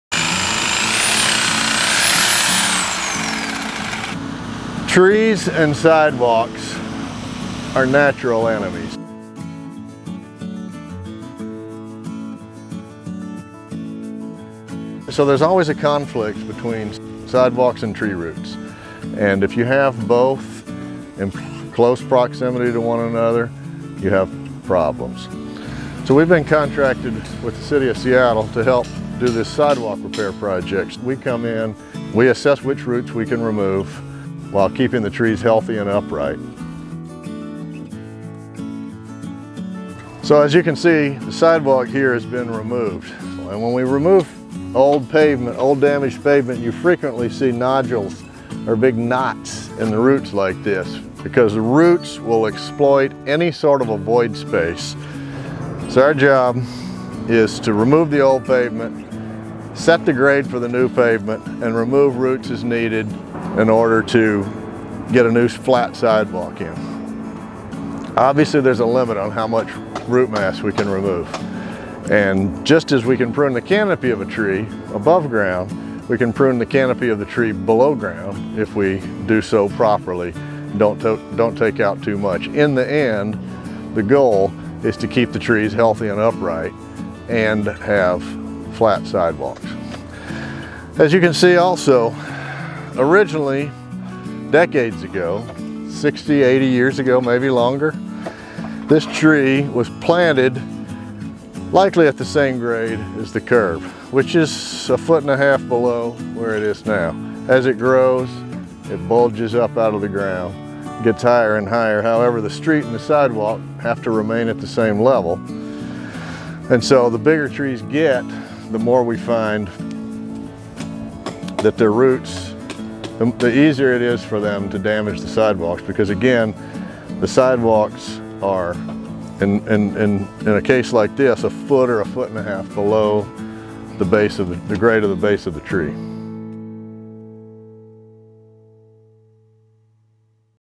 Tree Roots vs Sidewalks - Sidewalk Repair Project in Seattle, Washington
Tree Roots and Sidewalks are natural enemies! Whenever the two are in close proximity, there are going to be problems. We were called into a project in the Capitol Hill neighborhood for this sidewalk repair project. Our job was to assess what roots we can remove while keeping the tree healthy and upright. When we remove old damaged pavement, you can learn a lot about a tree and its history (some would call struggle) in growing through our construction. Just as we can prune the canopy of the tree above ground, we can safely prune the tree underground.
In the video, the sidewalk has been removed to reveal the roots of the sets of trees. In these particular trees, they have some large knots in them, which stems from the roots exploiting empty space in the soil.
By removing some of the roots on these trees, we are able to replace the sidewalk that will be at the same grade as the street curb.
Decades ago, when this sidewalk was installed, they were parallel with street, now they are about a foot to a foot and a half above the street line.